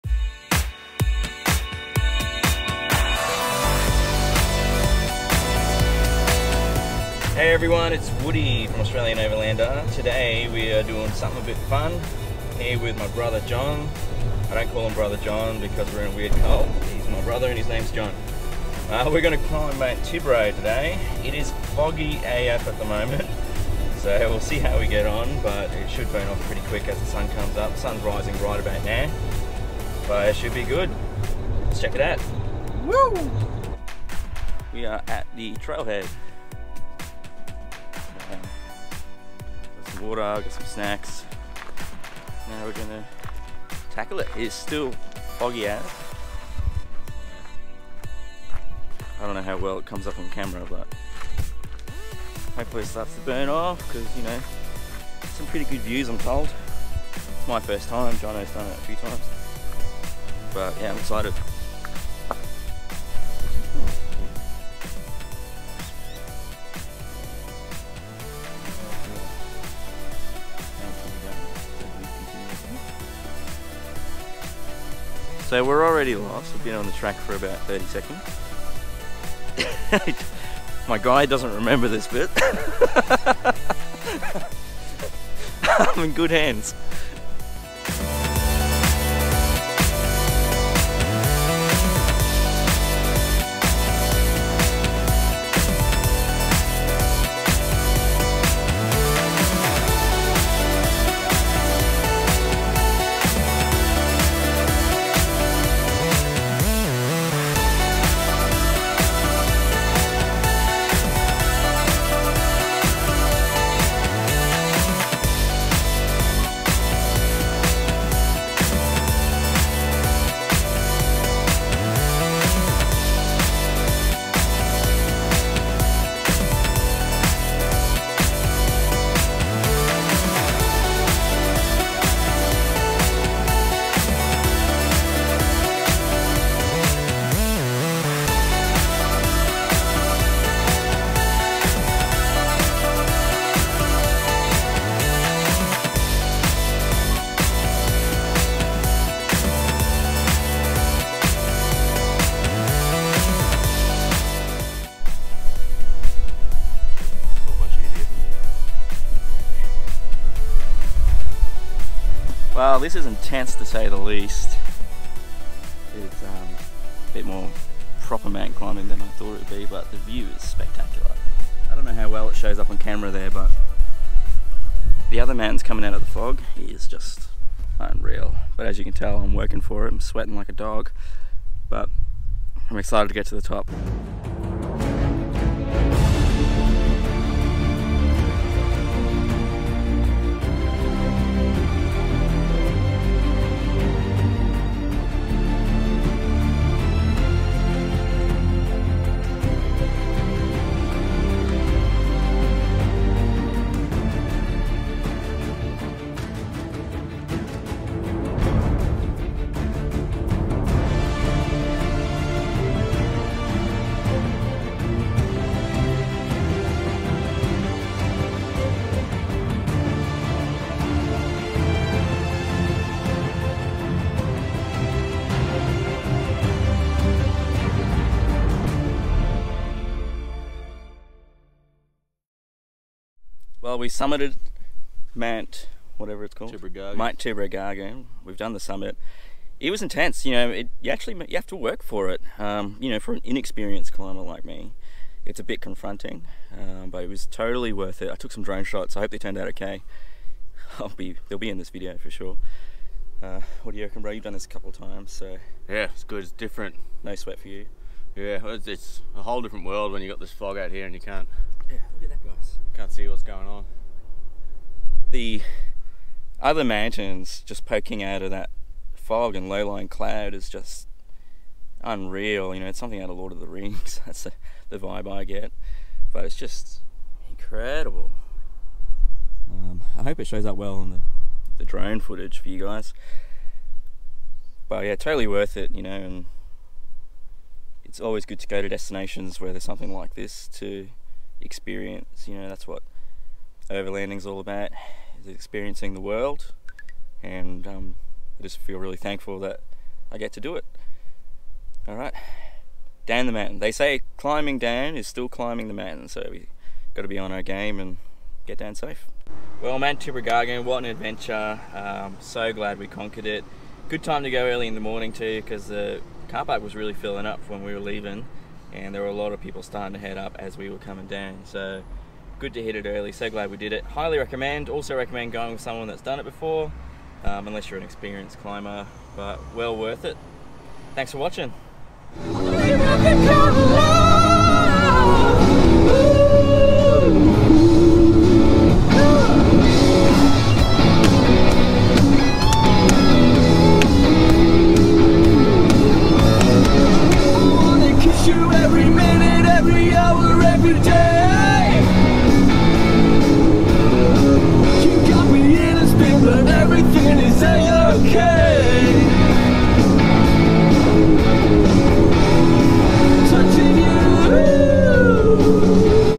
Mt Tibrogargan - AUSTRALIAN OVERLANDER - EPIC DRONE FOOTAGE
Woody and John tackle Mt Tibrogargan with an epic climb to the summit in some intense weather. Woody tackles his fears and is rewarded with some incredible views with low lying cloud to make some amazing photos and video.